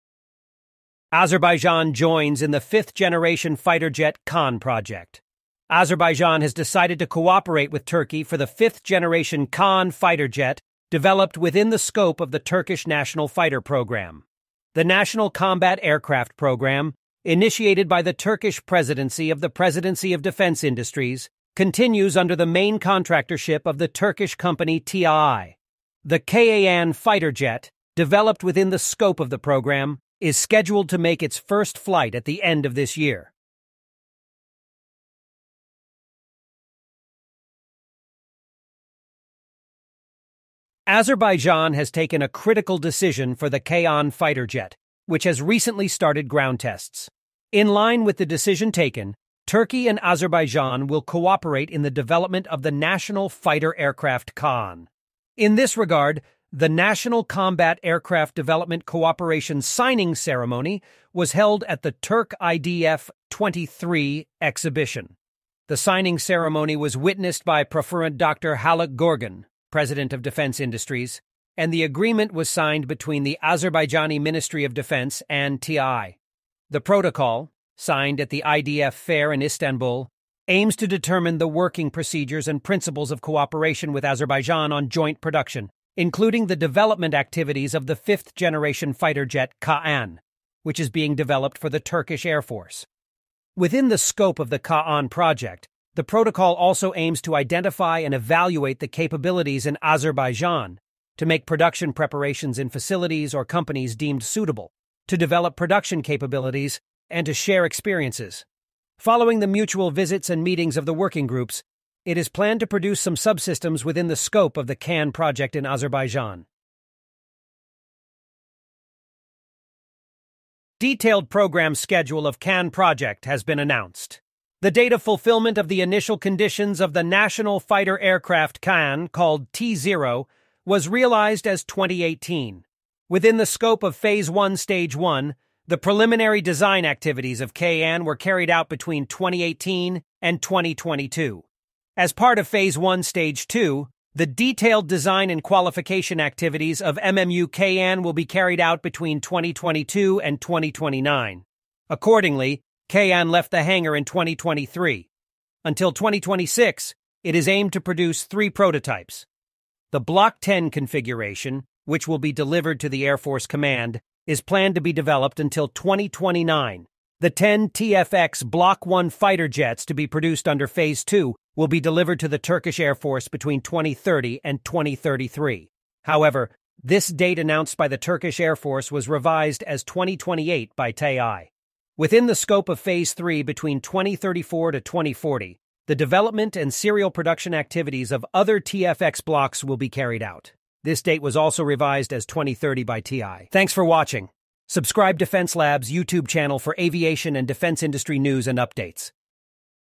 Azerbaijan Joins Turkiye's Kaan Project
Azerbaijan Joins in the Fifth Generation Fighter Jet Kaan Project.
Azerbaijan has decided to cooperate with Turkey for the fifth-generation KAAN fighter jet developed within the scope of the Turkish National Fighter Program.
The National Combat Aircraft Program, initiated by the Turkish Presidency of the Presidency of Defense Industries, continues under the main contractorship of the Turkish Company TAI. The KAAN Fighter Jet, developed within the scope of the program, is scheduled to make its first flight at the end of this year.

0:00 Azerbaijan Joins Kaan Project
1:57 Detailed Program Schedule of  KAAN Project